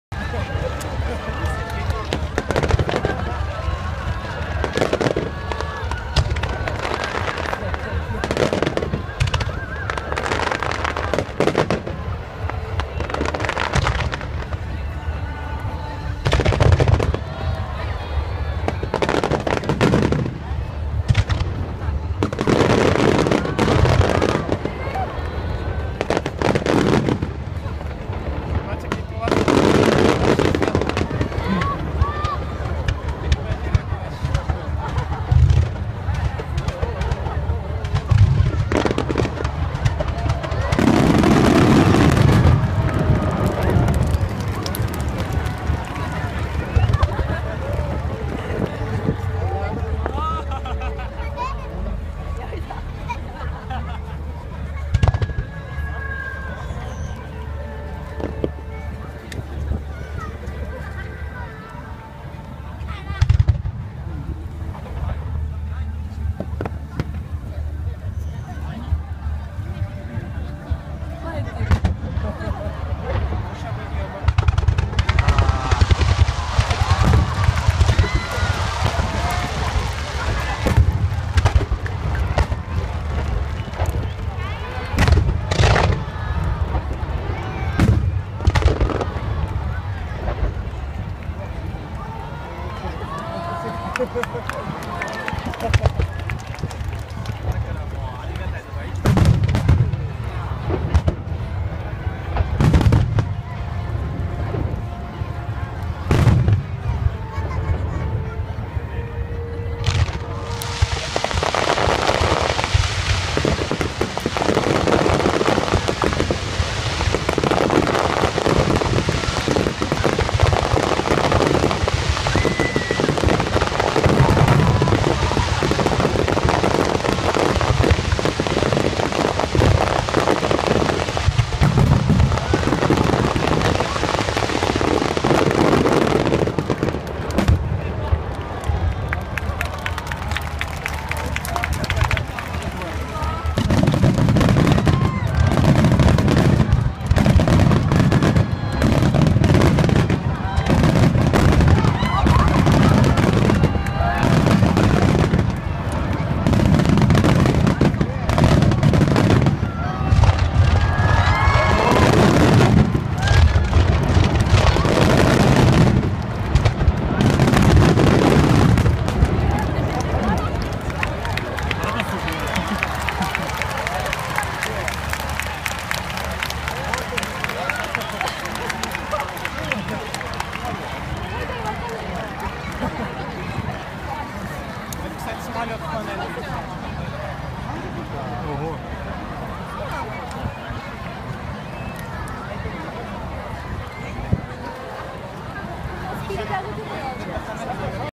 Салют в Токио 2014.08.15
Небольшой кусок салюта, который был в Токио 15 августа 2014 года.